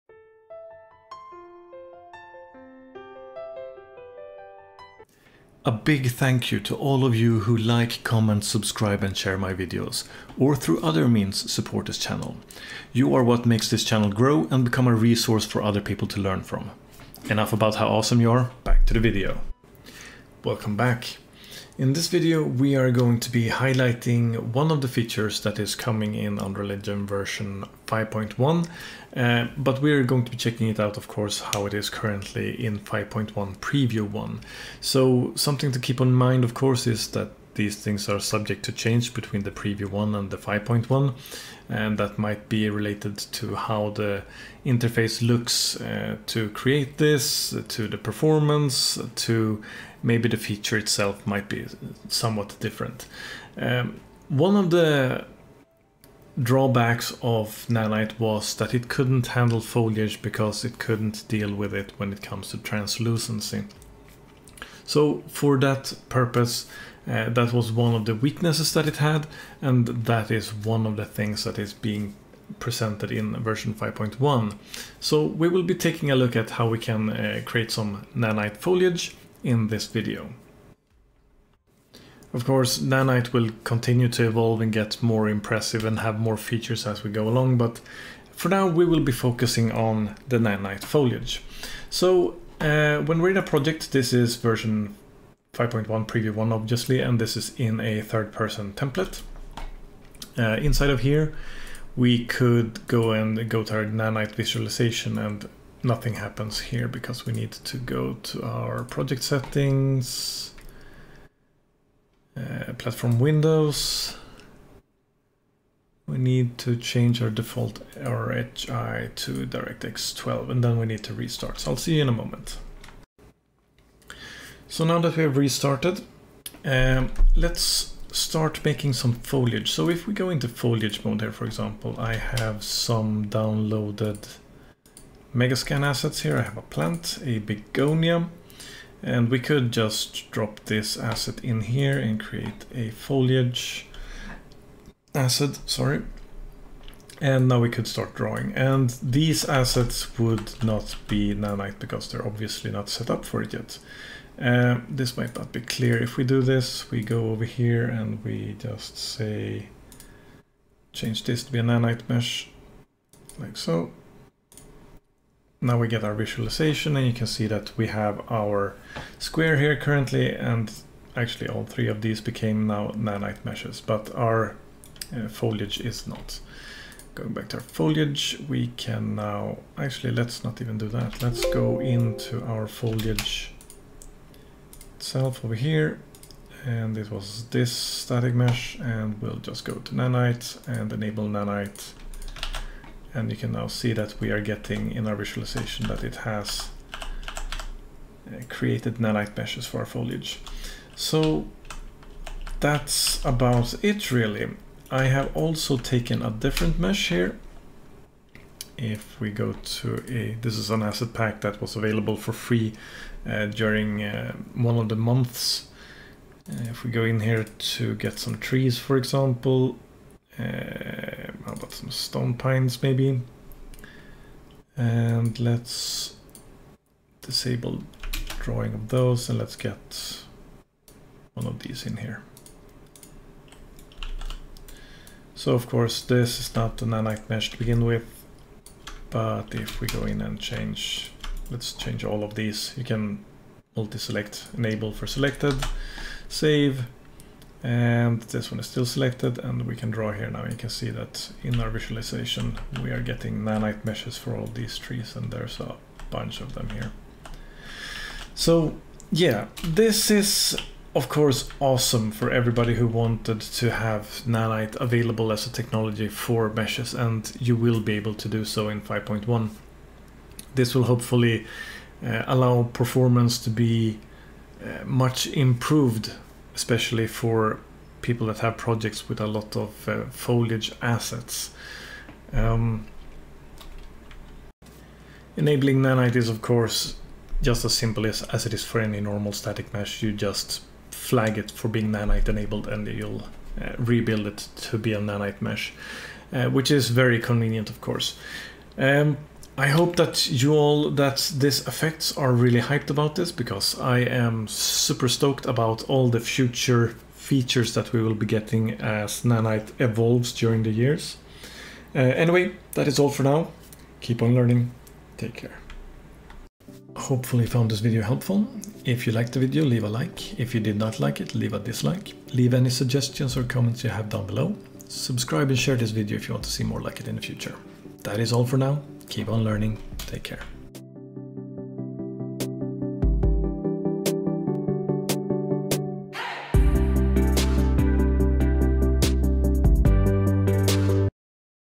Unreal Engine 5.1 Preview 1 highlights: Nanite foliage
Unreal engine 5.1 preview 1 is now available to download. Please note that this is mainly for testing and should not be used for production projects. In this video we are highlighing Nanite foliage.